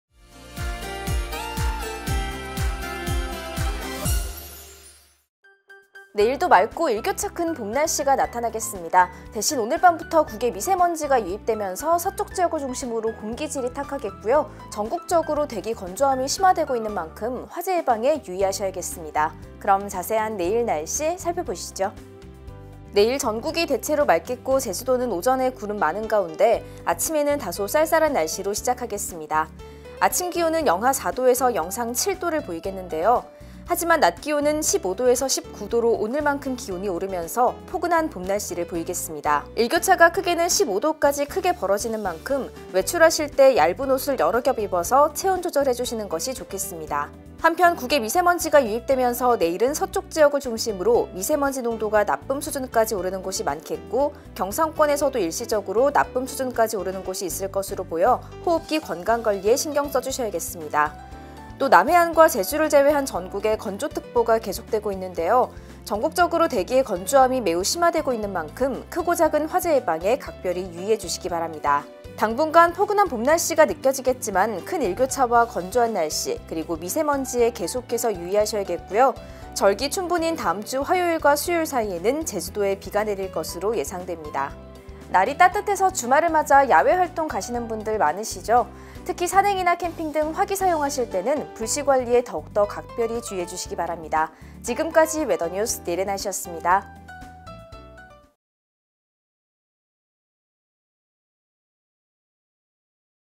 [웨더뉴스] 내일의 날씨 (03월 19일)
내일도 맑고 일교차 큰 봄 날씨가 나타나겠습니다.

오늘 밤부터 국외 미세먼지가 유입되면서 서쪽 지역을 중심으로 공기 질이 탁하겠고요.
전국적으로 대기 건조함이 심화되고 있는 만큼 화재 예방에 유의하셔야겠습니다.

오늘의 캐스터 : 이현서

더욱 자세한 날씨가 궁금하다면 웨더뉴스 날씨 정보 보러 가기